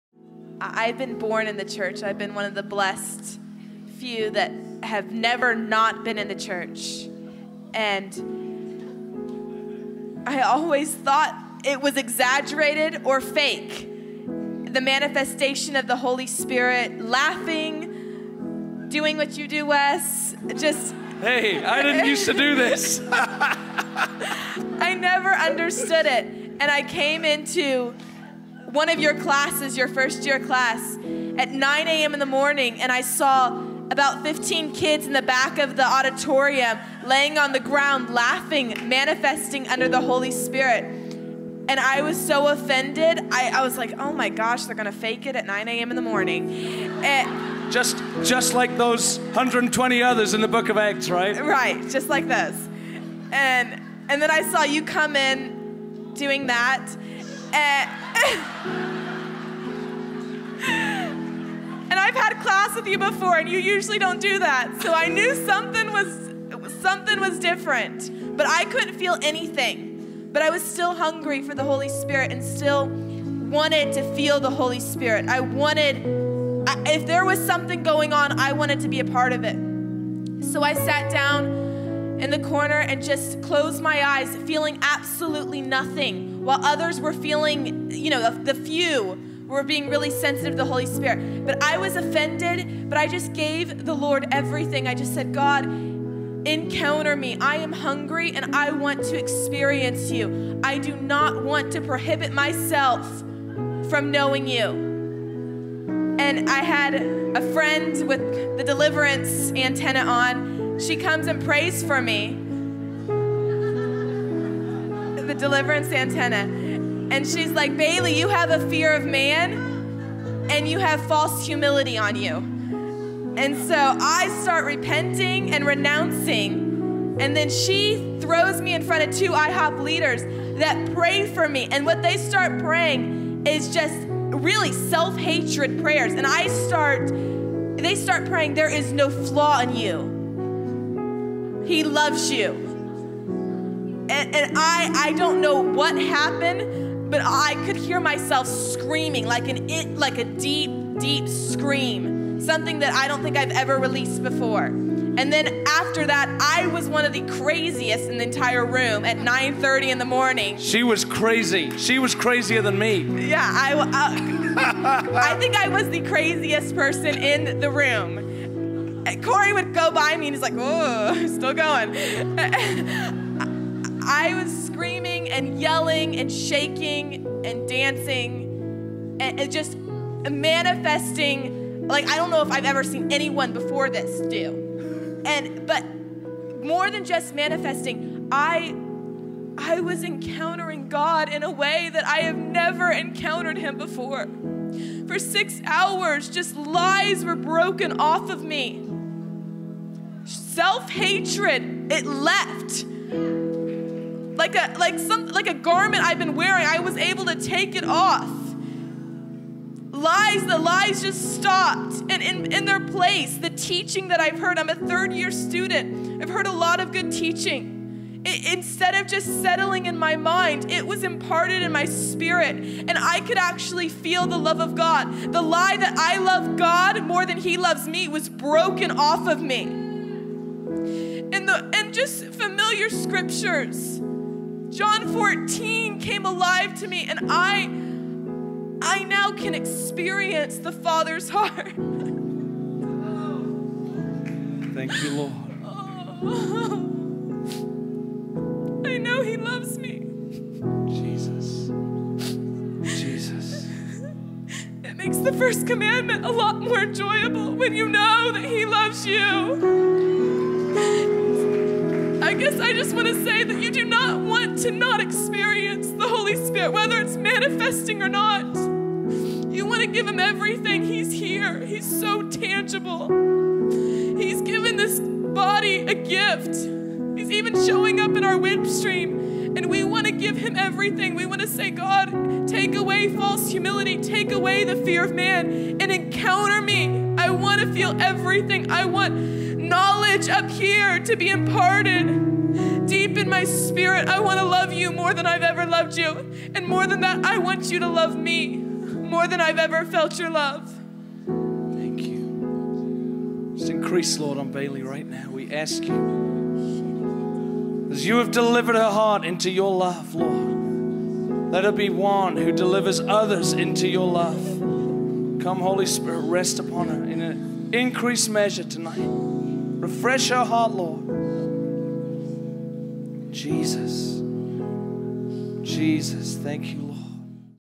Bailey's Testimony - Freed from fear of man and offense towards God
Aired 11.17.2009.

IHOP.org/Watch.